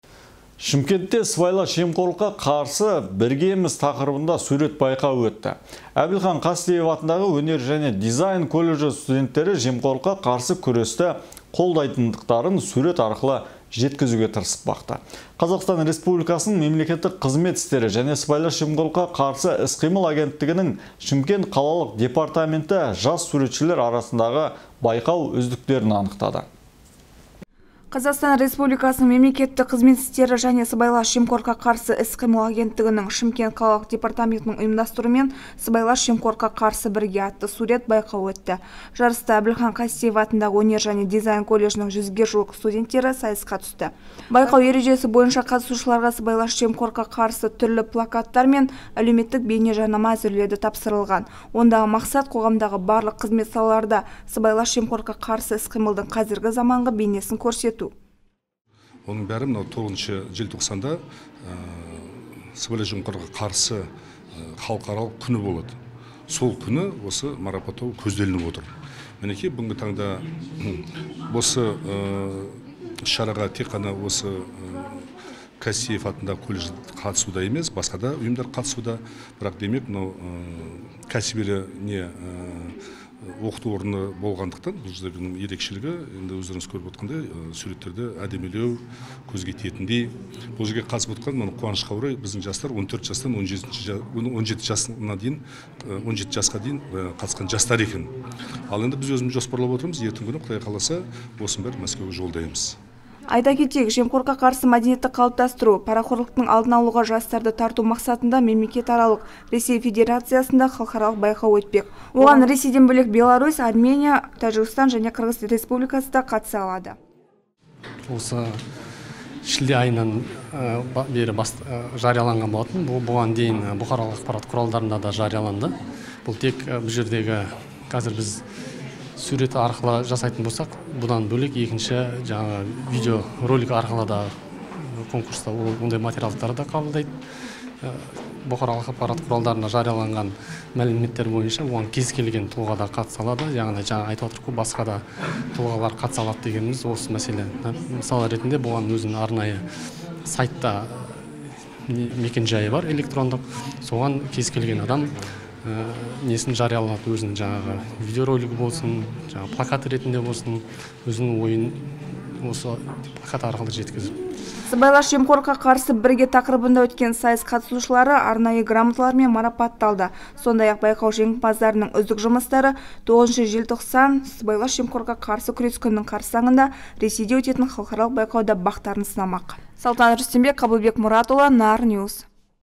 «СЫБАЙЛАС ЖЕМҚОРЛЫҚҚА ҚАРСЫ БІРГЕ!» АТТЫ СУРЕТ БАЙҚАУЫ ӨТТІ
Шымкентте «Сыбайлас жемқорлыққа қарсы бірге» тақырыбында сурет байқауы өтті.  Әбілхан Қастеев атындағы өнер және дизайн колледжі  студенттері жемқорлыққа қарсы күресті қолдайтындарын сурет арқылы жеткізді. Қазақстан Республикасының Мемлекеттік қызмет істері және сыбайлас жемқорлыққа қарсы іс-қимыл Агенттігінің Шымкент қалалық департаменті жас суретшілер арасындағы байқау үздіктерін анықтады.